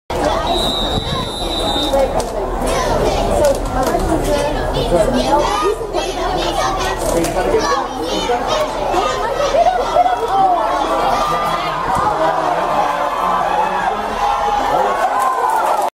Kent Knights - Arthur Shaw Gets Hit of the Game Against SWAC
Knights linebacker Arthur Shaw gets the hit of the game in their contest against SWAC, finally stopping #24, who'd been the only player the Knights couldn't stop that day.

It was a textbook, open field tackle, as the ball carrier didn't even see him coming until it was too late.

They had no more problems with big 24 after this hit, however, as he was out for the remainder of the game.